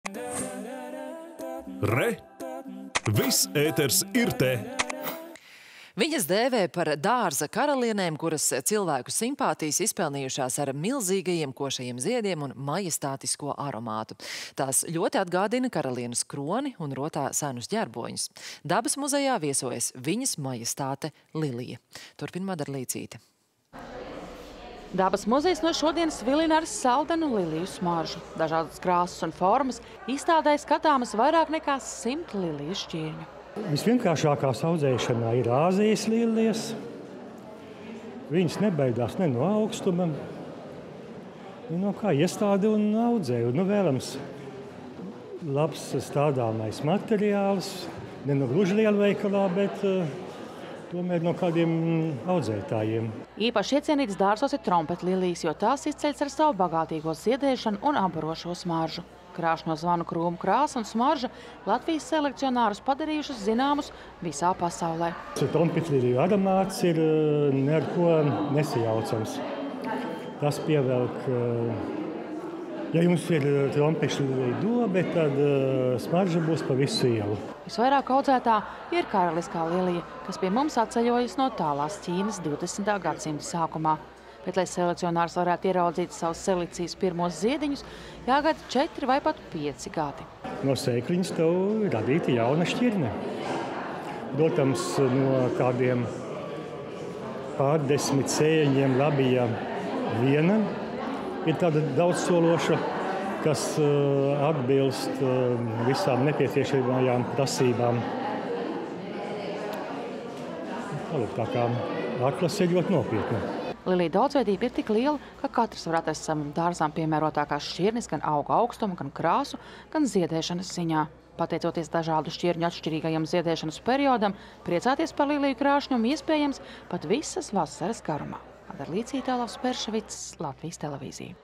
Dabas muzejā aplūkojamas krāšņās lilijas
Viņas dēvē par dārza karalienēm, kuras cilvēku simpātijas izpelnījušās ar milzīgajiem, košajiem ziediem un majestātisko aromātu. Tās ļoti atgādina karalienes kroni un rotā senus ģerboņus. Dabas muzejā viesojas viņas majestāte -lilija. Turpina Madara Līcīte.